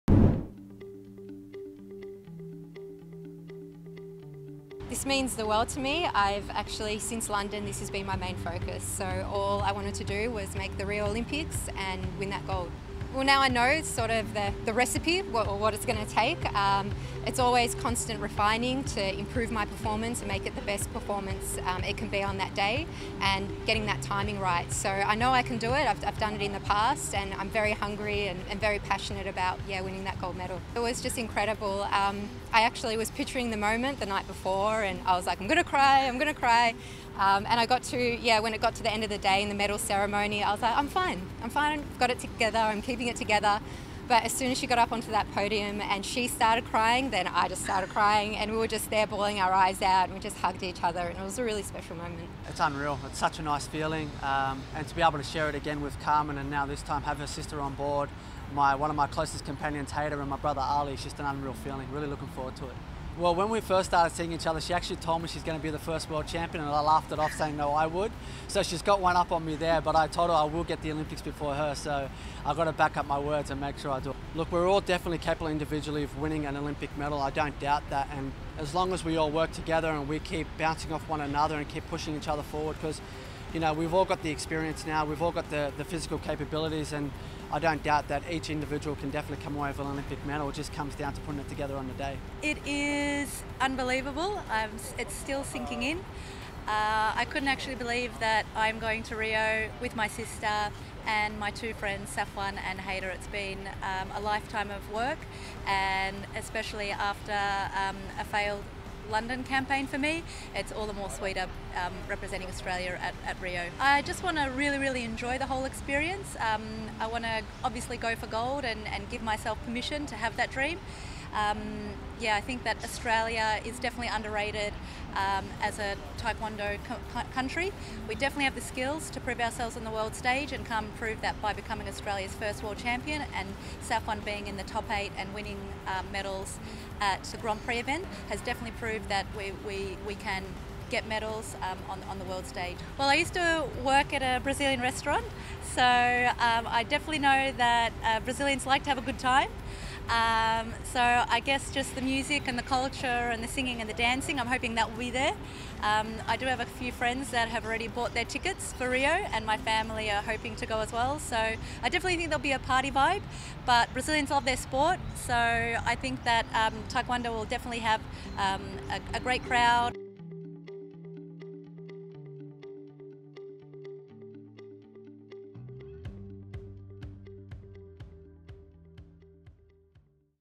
Medals on the minds of Aussie taekwondo athletes
Carmen Marton, Safwan Khailil and Caroline Marton dicuss their selection to the Australia Olympic Team and their ambitions for Rio 2016.